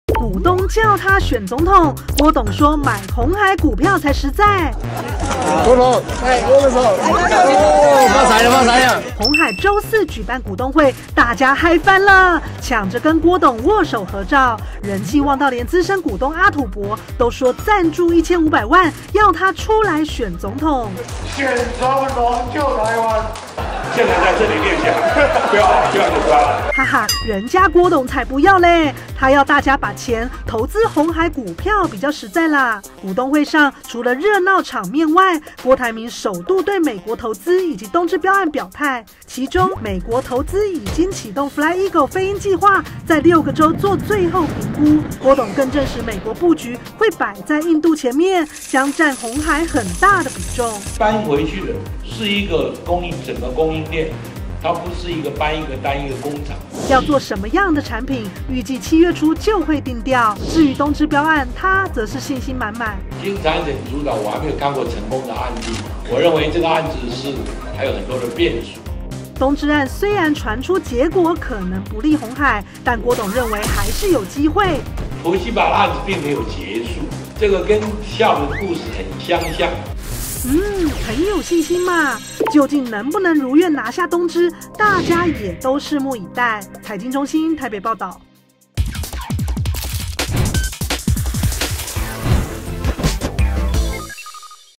郭董不想當總統：只願兩岸和平不打仗 | 台灣蘋果日報
鴻海今日舉行股東大會，小股東發言踴躍，資深股民阿土伯說，要加碼贊助郭台銘選總統，郭台銘表示，可以把錢投資鴻海股票就好；他也透露，對美投資內部正啟動Fly eagle飛鷹計劃，預計7月底、8月初會進行最後定調，此外，在股東會結束後，郭台銘下午2點半左右，與媒體舉行見面會，親自對中外媒體說明，媒體主要聚焦美國投資及東芝記憶體競標案。